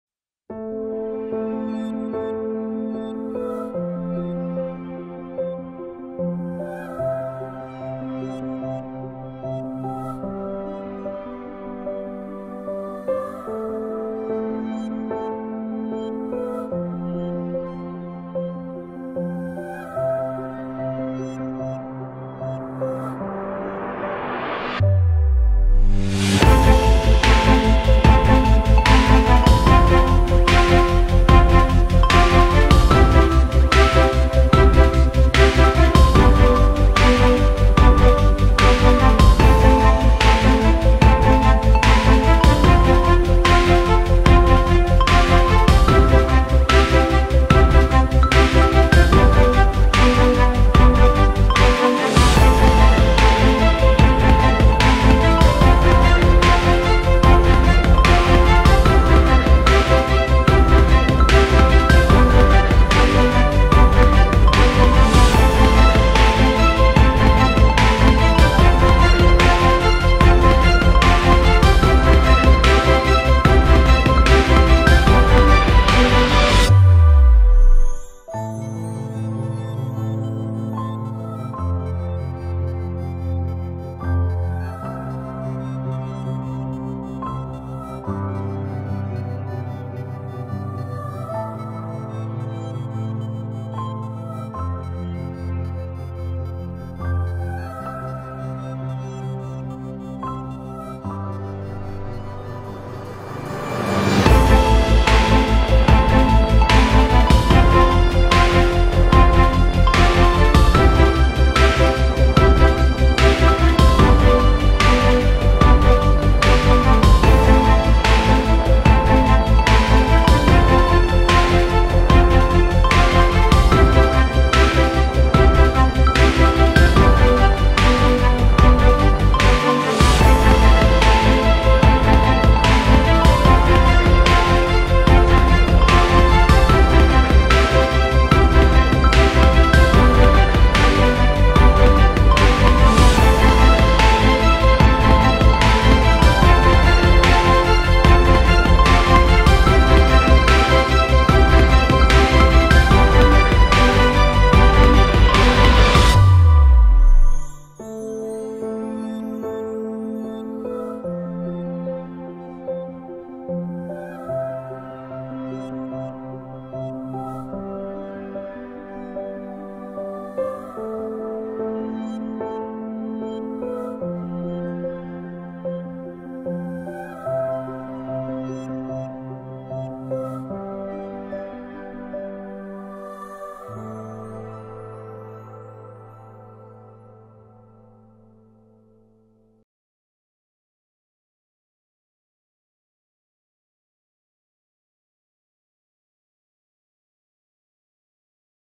La Corse vue du ciel - Mavic Pro
Divers paysages corses. La plupart des images ont été prises au village de Pino et à Porto Ota avec un drone Mavic Pro.